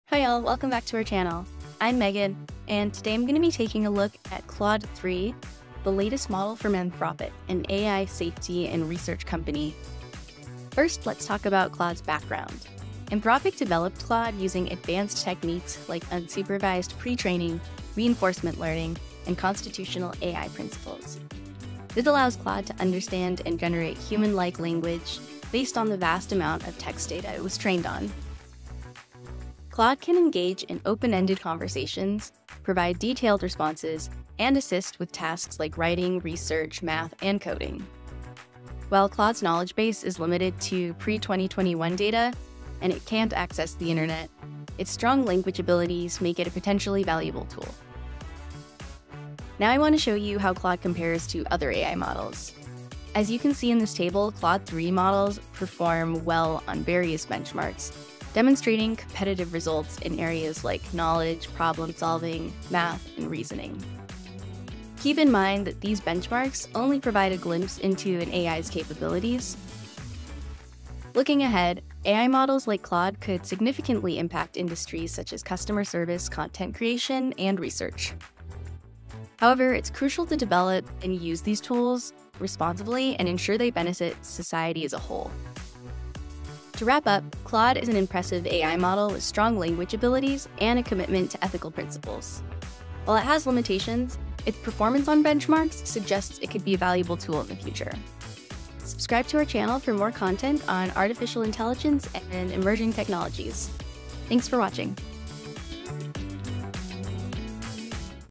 An overview of Anthropic AI's Claude 3
Megan.AI provides an overview of Anthropic's latest LLM, Claude3, which outperforms Gemini and GPT-4 on various benchmarks. Take a look at some of the benchmarks and learn more about the AI safety and research company behind it.